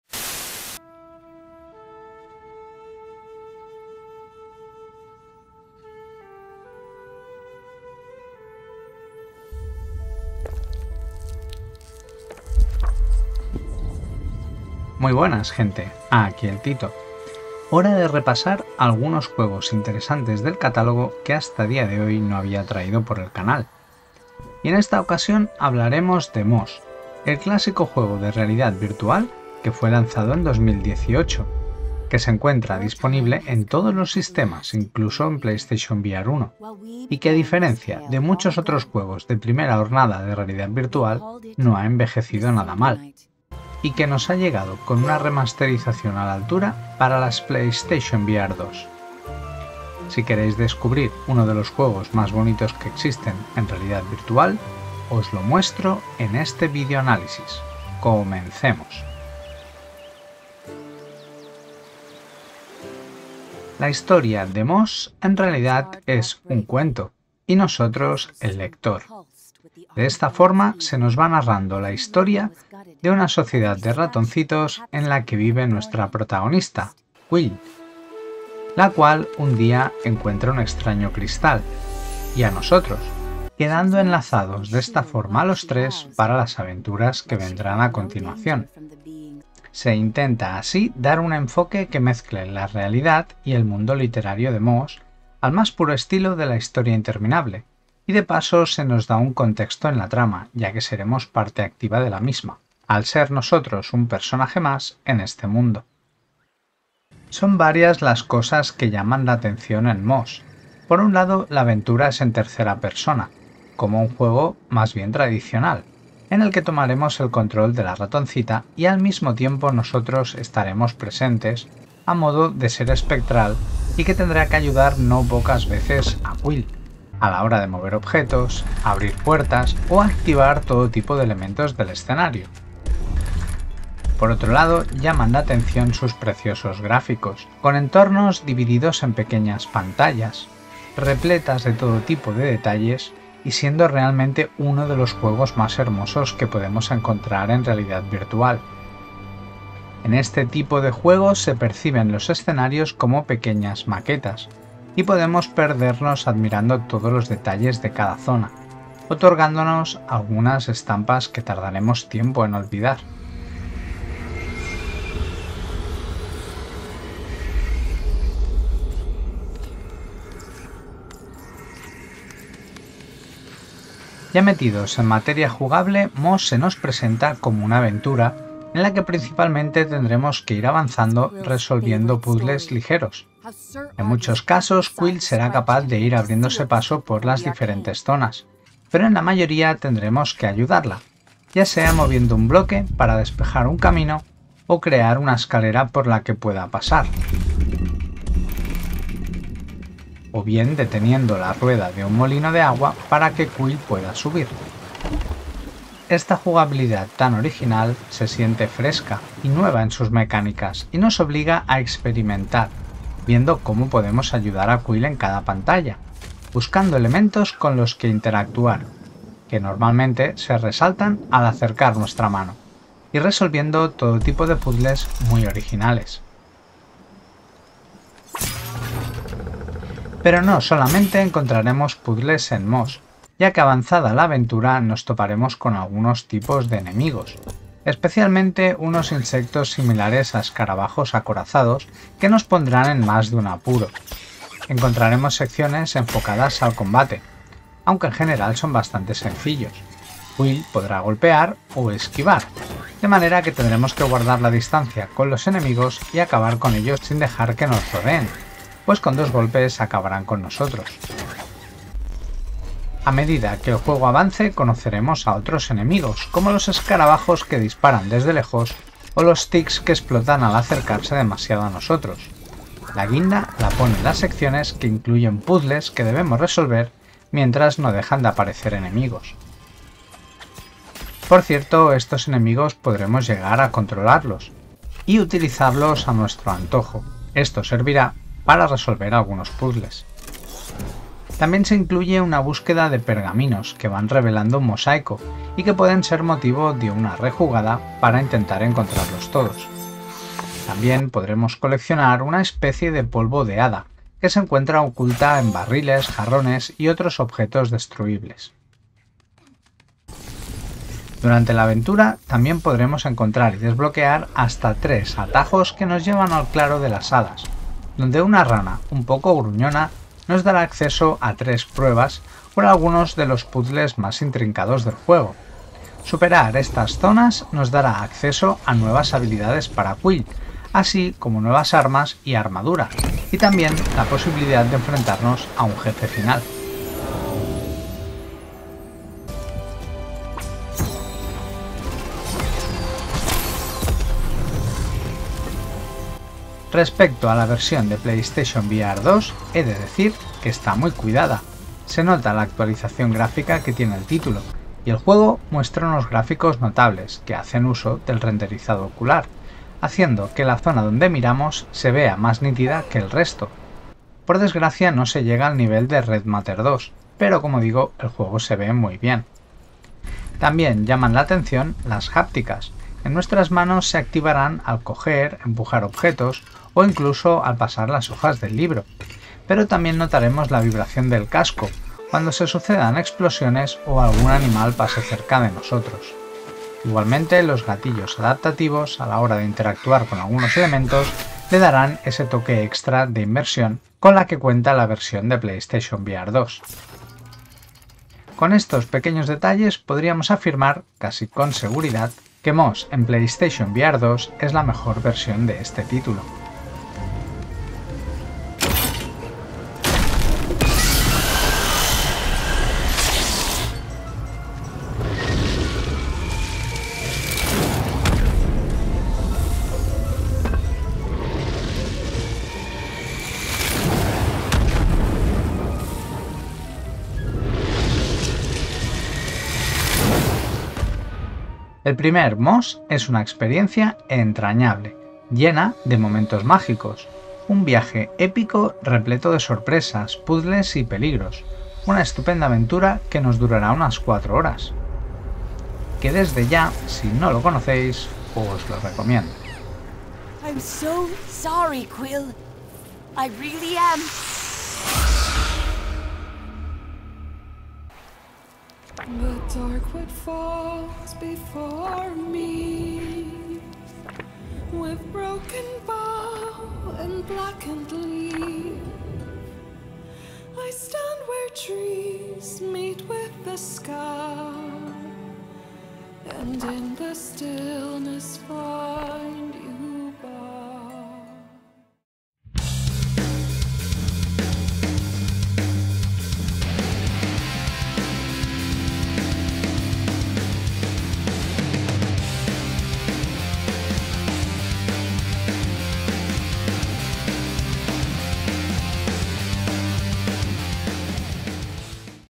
MOSS - PSVR2 ANALISIS
Hoy traemos al canal Moss, la genial aventura de la ratoncita Quill, que se embarca en una misión tan bonita como peligrosa!

Moss™ es un juego de ingenio, acción y aventura, una franquicia de Polyarc para la plataforma PS VR. Toma componentes clásicos de grandes juegos (personajes interesantes, combate atrapante y exploración de un mundo cautivante) y los combina con las emocionantes oportunidades de la realidad virtual.

En Moss, conocerán a Quill, una ratoncita con grandes sueños que sobrepasan los confines de su vida tranquila. Un día, explorando, Quill encuentra una piedra misteriosa y una magia ancestral se despierta. Rápidamente, se desencadenan eventos dramáticos cuando un mal lejano extiende su alcance. Ahora su tío está en grave peligro y Quill debe embarcarse en un viaje épico... y te necesita. Juntos, viajarán a reinos olvidados, resolverán situaciones difíciles y lucharán con enemigos peligrosos. En soledad, nadie puede superar lo que está por enfrentar. Pero unidos, tal vez derroten al más oscuro de los villanos. Sumérgete en el mundo de Moss y ayuda a escribir la historia de esta nueva heroína.